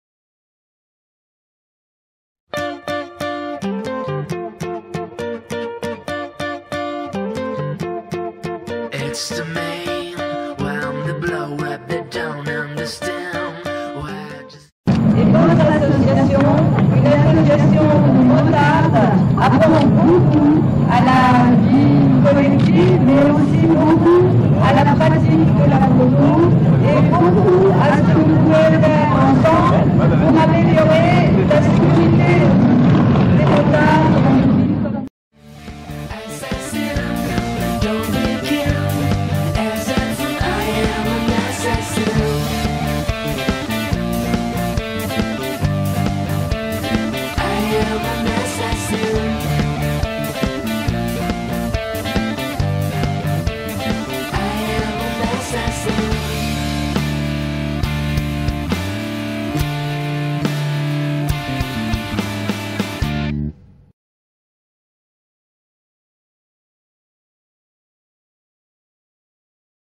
Mme Anne Hidalgo et la moto, de 2014 à 2016
En mars 2014, Anne Hidalgo s'est exprimée très favorablement envers les motard(e)s lors de "Toutes en moto" Paris. En 2016, elle prévoit d'engager des interdits drastiques contre les 2 roues motorisés.